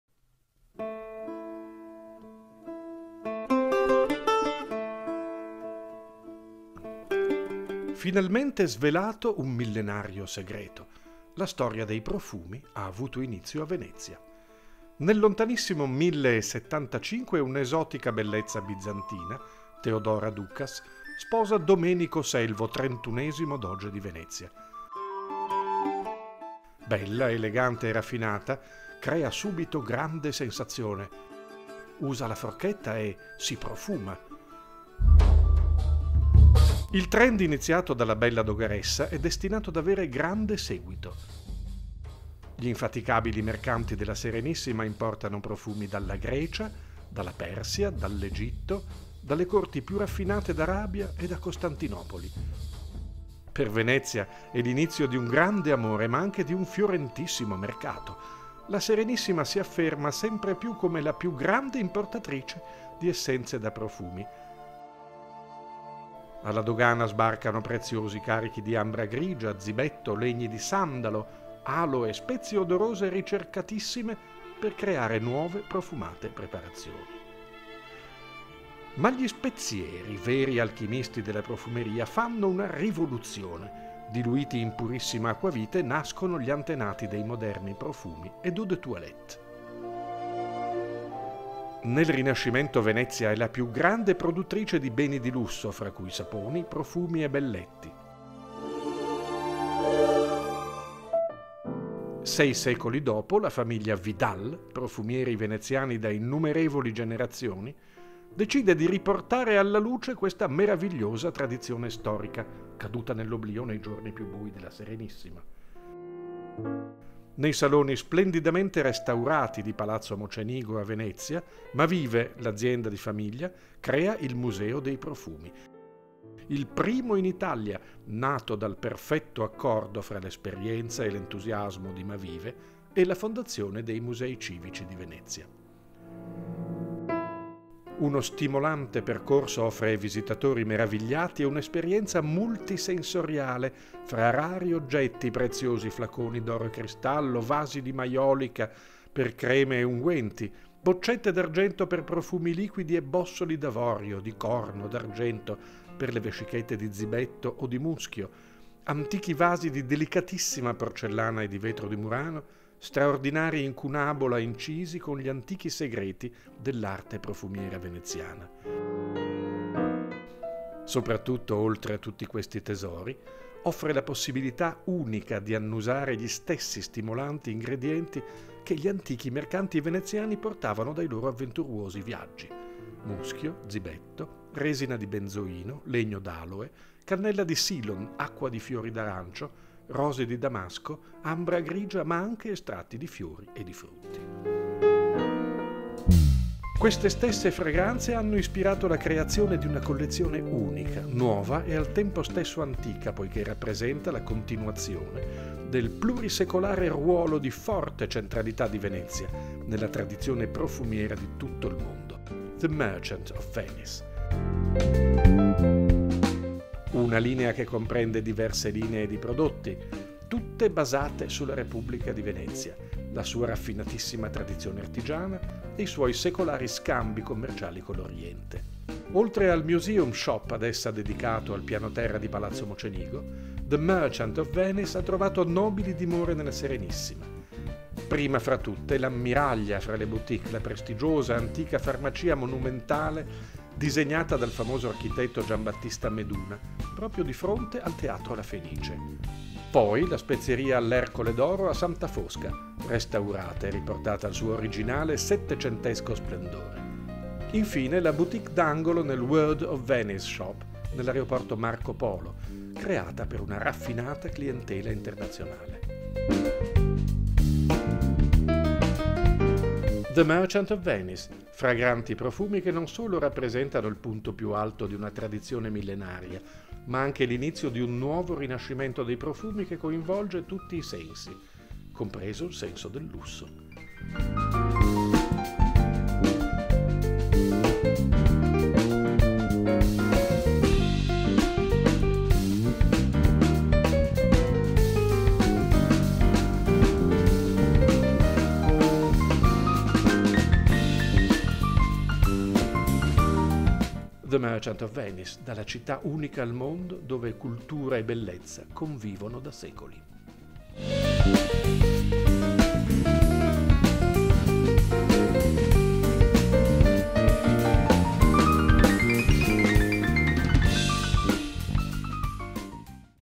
The Merchant of Venice
The Merchant of Venice è una linea strettamente legata a Venezia e alla sua storia: rimarca il ruolo di capostipite che la città ebbe nella tradizione profumiera mondiale e si articola in diverse linee di prodotto ispirate alla Serenissima, al suo raffinatissimo artigianato e al suo secolare commercio con l’Oriente.